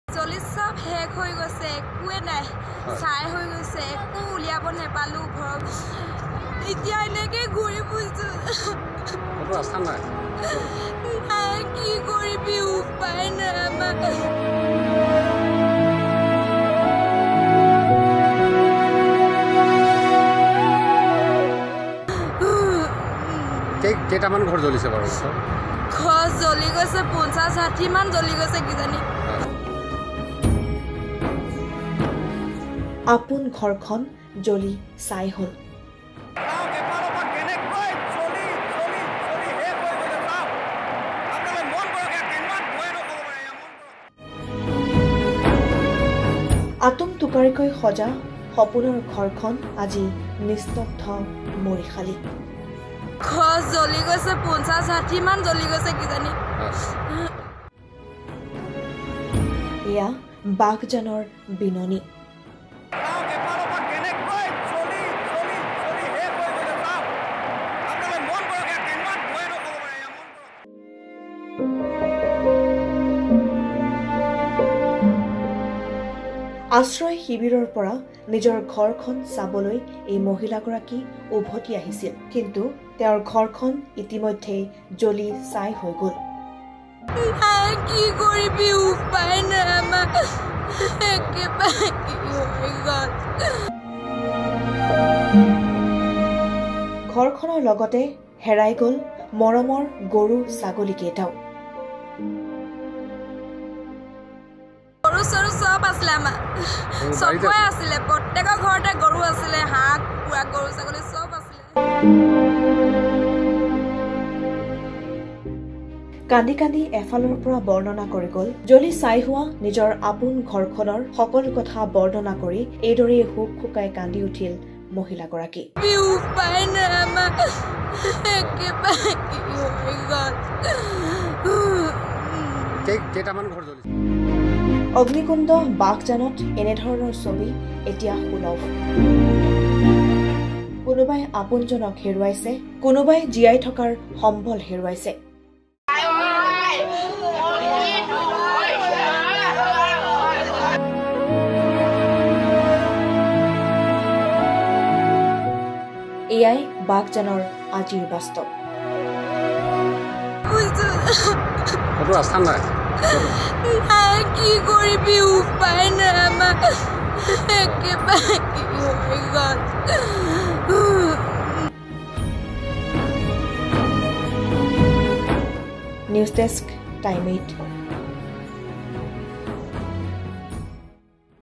"কি কৰিবি অ , সকলো শেষ হৈ গল, ঘৰ নাই ছাই হৈ গল অ,"
জ্বলি ছাই হোৱা ঘৰৰ সন্মুখত বিননী , বাঘজানৰ অইল খাদেই কাল,
অমানিশা হৈ নামি আহিল বহু পৰিয়াললৈ,